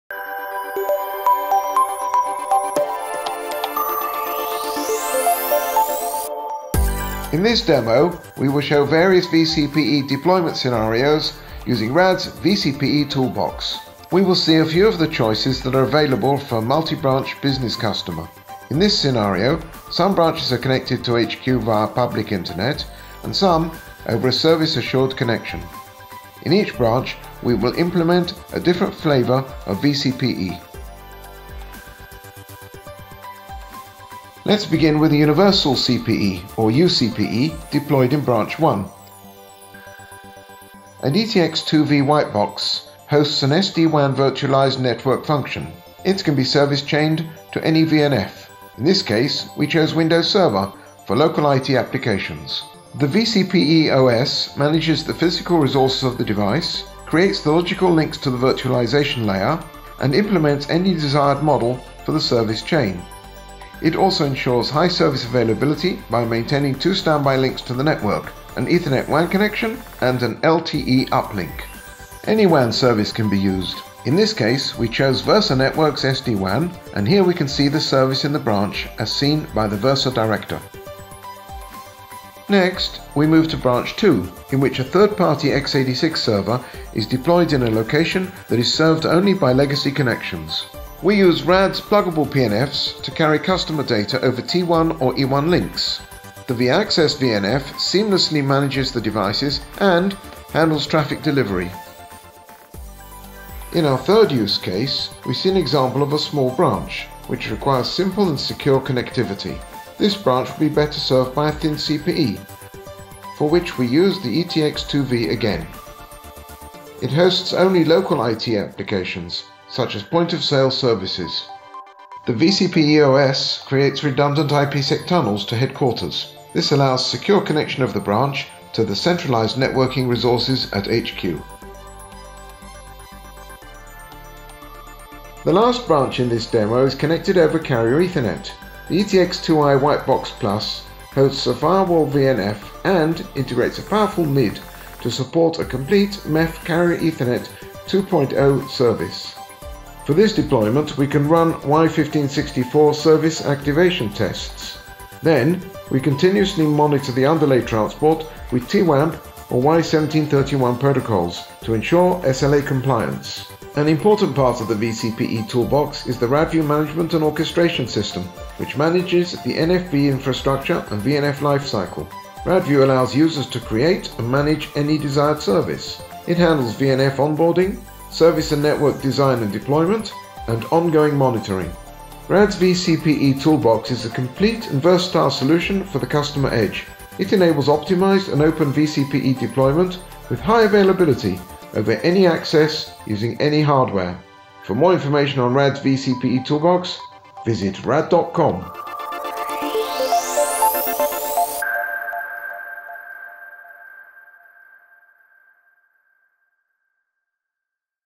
RAD’s vCPE Toolbox Demo
This demo shows various vCPE flavors and deployment scenarios using RAD’s vCPE Toolbox.

We will see a few of the choices that are available for a multi-branch business customer. In this scenario, some branches are connected to HQ via public internet and some, over a service assured connection.

They use a variety of uplink connections. In each branch, we will implement a different flavor of vCPE.

Let’s begin with a universal CPE, or uCPE, deployed in branch 1.

An ETX-2v white box hosts an SD-WAN virtualized network function. It can be service-chained to any VNF. In this case, we chose WINDOWS server for local IT applications.

The vCPE-OS manages the physical resources of the device, creates the logical links to the virtualization layer and implements any desired model for the service chain.

It also ensures high service availability by maintaining two standby links to the network – an Ethernet WAN connection and an LTE uplink.

Any WAN service can be used. In this case, we chose Versa Networks’ SD-WAN, and here we can see the service in the branch as seen by the Versa Director.

Next, we move to branch #2, in which a third-party x86 server is deployed in a location that is served only by legacy connections.

We use RAD’s pluggable PNFs to carry customer data over T1 or E1 links, and the vAccess VNF seamlessly manages the devices AND handles traffic delivery.

In our third use case, we see an example of a small branch, which requires only simple and secure connectivity.  On-premise network function virtualization is too costly and not necessary.

This branch will be better served by a thin CPE, for which we use the ETX-2v again. It only hosts local IT applications, such as point of sale services.

The vCPE-OS creates redundant IPsec tunnels to headquarters. This allows secure connection of the branch to the centralized networking resources at HQ.

The last branch in this demo is connected over Carrier Ethernet. The ETX-2i whitebox+ hosts a firewall VNF AND integrates a powerful NID to support a complete MEF Carrier Ethernet 2.0 service.

For this deployment, we can run Y.1564 service activation tests.

Then, we continuously monitor the underlay transport with TWAMP or Y.1731 protocols, to ensure SLA compliance.

An important part of the vCPE Toolbox is the RADview management and orchestration system, which manages the NFV infrastructure and VNF lifecycle.

RADview allows users to create and manage any desired service. It handles VNF on-boarding, service and network design and deployment, and on-going monitoring.

RAD’s vCPE Toolbox is a complete and versatile solution for the customer edge. It enables optimized and open vCPE deployment with High Availability over any access, using any hardware.